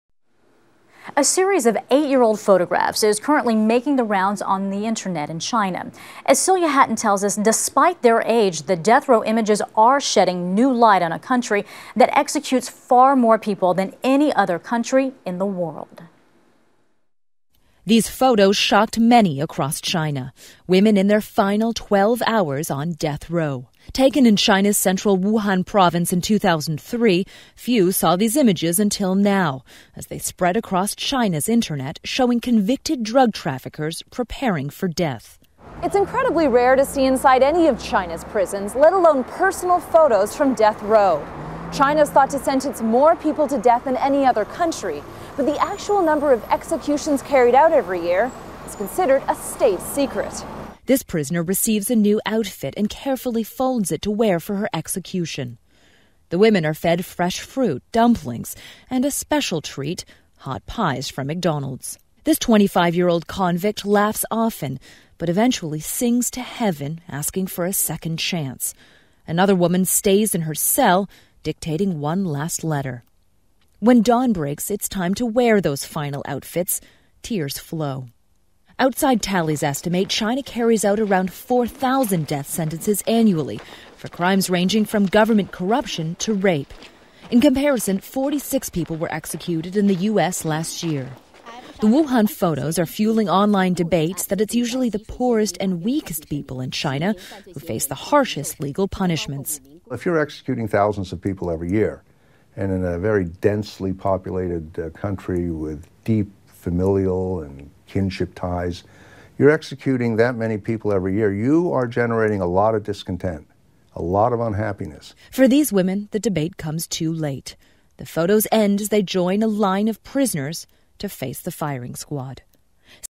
Chinese executions exposed by rare photos
A series of photographs is currently making the rounds on the internet in China. As Celia Hatton reports, the death row images are shedding new light on a country that executes far more people than any other country in the world.